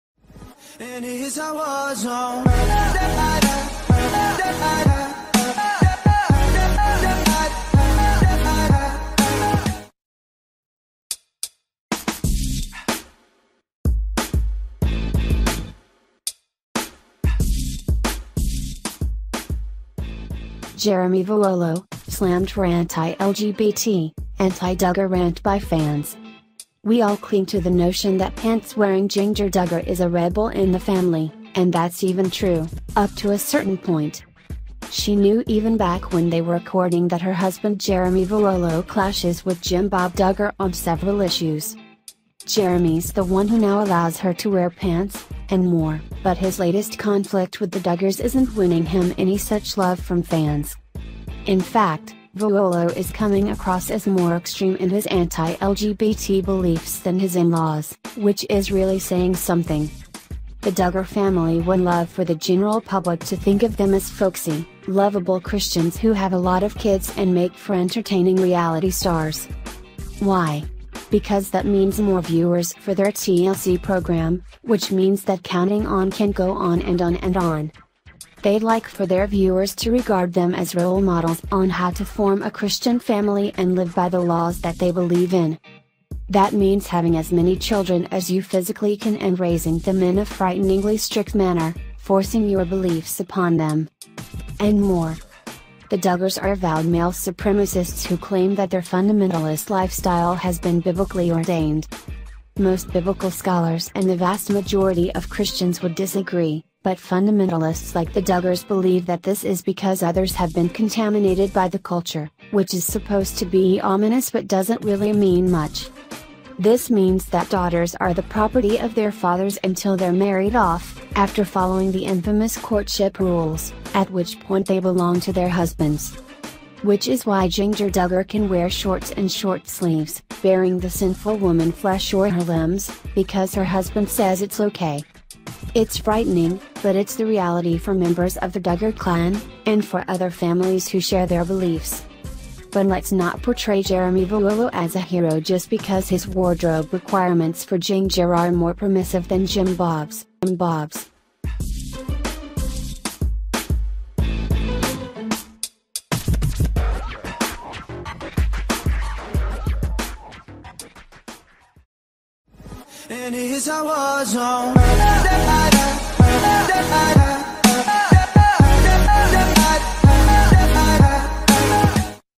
"ANTI-DUGGAR RANT BY FANS" Jingger Duggar Husband Jeremy Vuolo "SLAMMED"
RealityTVserieS

Jeremy Vuolo: Slammed For Anti-LGBT, Anti-Duggar Rant By Fans

We all cling to the notion that pants-wearing Jinger Duggar is a rebel in the family, and that's even true ... up to a certain point.

She knew even back when they were courting that her husband Jeremy Vuolo clashes with Jim Bob Duggar on several issues.

Jeremy's the one who now allows her to wear pants, and more, but his latest conflict with the Duggars isn't winning him any such love from fans.

In fact, Vuolo is coming across as more extreme in his anti-LGBT beliefs than his in-laws, which is really saying something.

The Duggar family would love for the general public to think of them as folksy, lovable Christians who have a lot of kids and make for entertaining reality stars.

Why? Because that means more viewers for their TLC program, which means that Counting On can go on and on and on.

They'd like for their viewers to regard them as role models on how to form a Christian family and live by the laws that they believe in.

That means having as many children as you physically can and raising them in a frighteningly strict manner, forcing your beliefs upon them.

And more.

The Duggars are avowed male supremacists who claim that their fundamentalist lifestyle has been Biblically ordained.

(Most Biblical scholars and the vast majority of Christians would disagree, but fundamentalists like the Duggars believe that this is because others have been "contaminated by the culture," which is supposed to be ominous but doesn't really mean much)

This means that daughters are the "property" of their fathers until they're married off (after following the infamous courtship rules), at which point they "belong" to their husbands.

Which is why Jinger Duggar can wear shorts and short sleeves, baring the sinful woman-flesh or her limbs - because her husband says it's okay.

It's frightening, but it's the reality for members of the Duggar clan - and for other families who share their beliefs.

But let's not portray Jeremy Vuolo as a hero just because his wardrobe requirements for Jinger are more permissive than Jim Bob's.